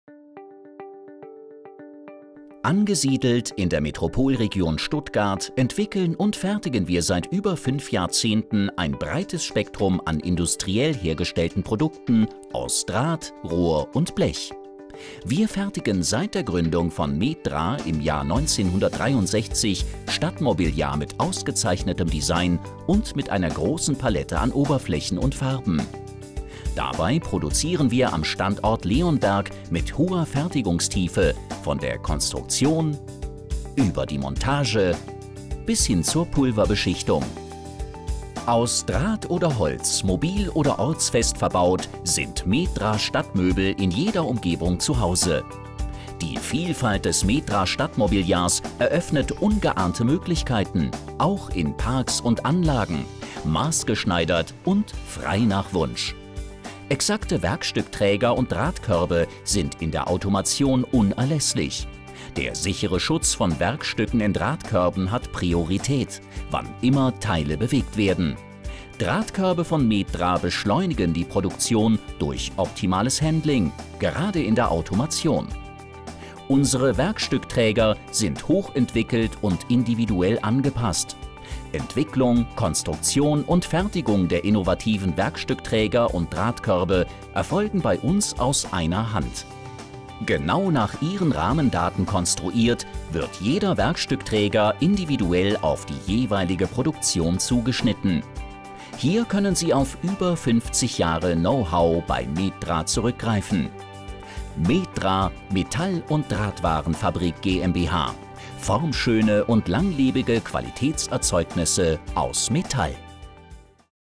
METDRA Metall und Drahtwarenfabrik GmbH | Unternehmensfilm
METDRA fertigt seit 1963 Stadtmobiliar mit ausgezeichnetem Design und mit einer großen Palette an Oberflächen und Farben. Aus Draht oder Holz, mobil oder ortsfest verbaut, sind METDRA-Stadtmöbel in jeder Umgebung zuhause. - METDRA Qualitätserzeugnisse aus Metall !